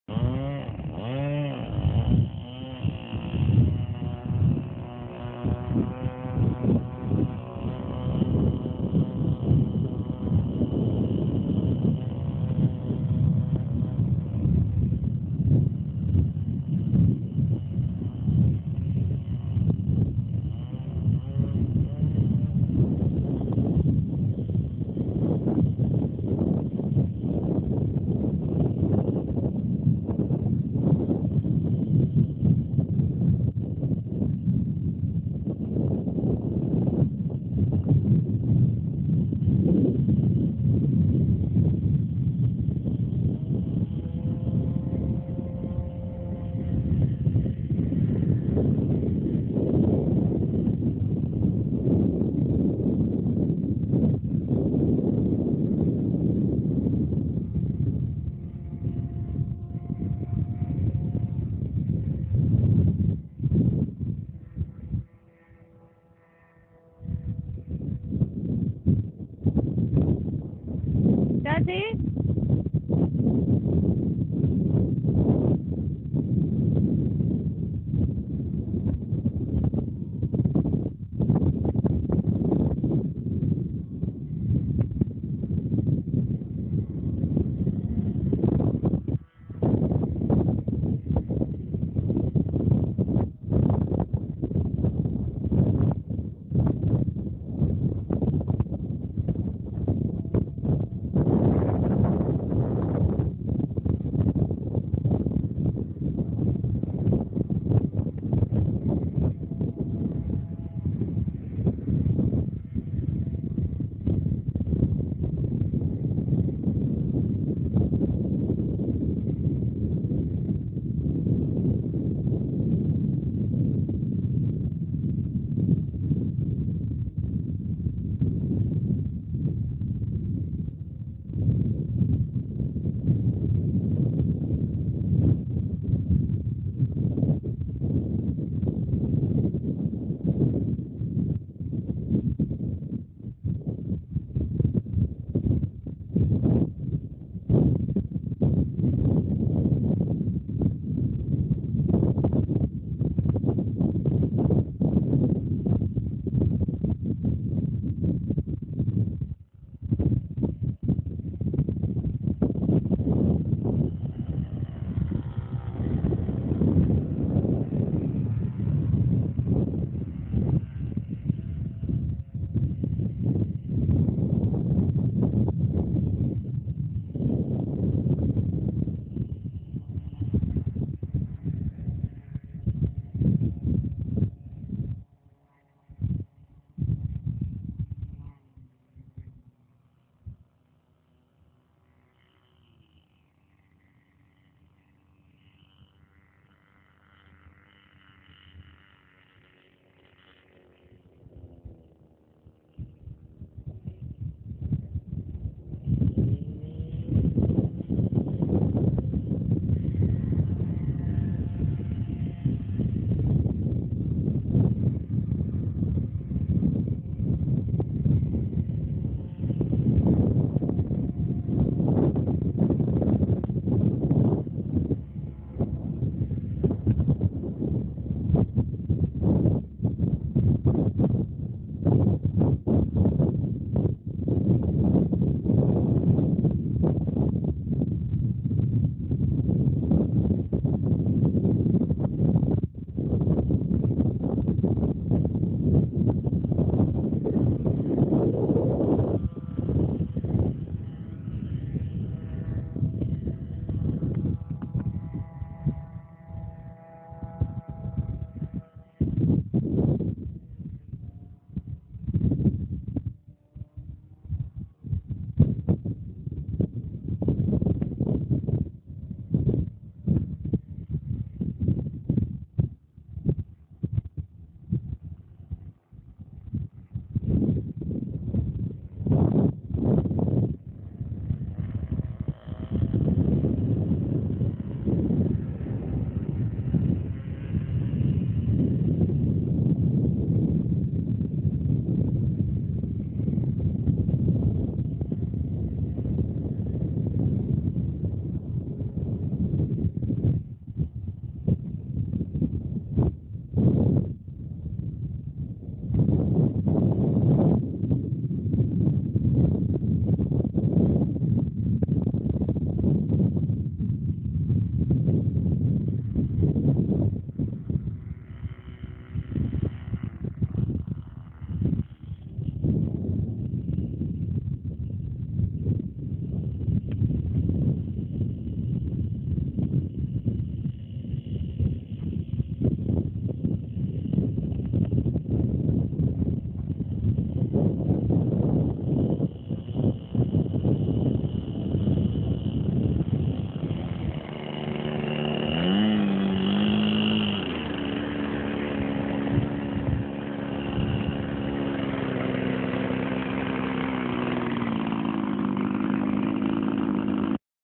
Dega Trainer 40
probando el Dega Trainer con motor O.S FP 25.. el modelo es un clon del verdadero Dega Trainer esta hecho con balsa de 2mm y reforzado con Varillas de 6x6mm terciado solo en tren de aterrizaje y para llamas...alas en foam masiso enchapado con lamina de 05mm en kiri balsa en borde de ataque y marginales un refuerzo de terciado desde el centro a la mitad de cada semi ala y un refuerzo perimetral de arriba y abajo en tela de fibra de vidrio timones huecos en balsa de 2mm y varillas de 3x10mm super liviano ...